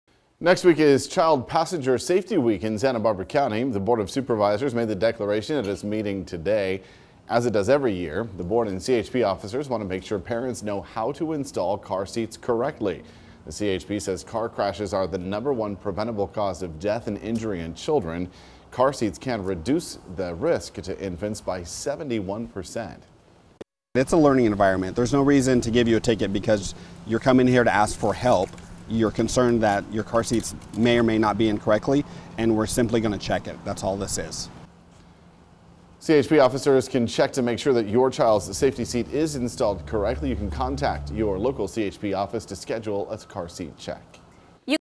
CHP urges parents to get their children's car seats checked for proper installation
Child Passenger Safety Week is this Sept. 23-29 and the local California Highway Patrol is already working to spread the word.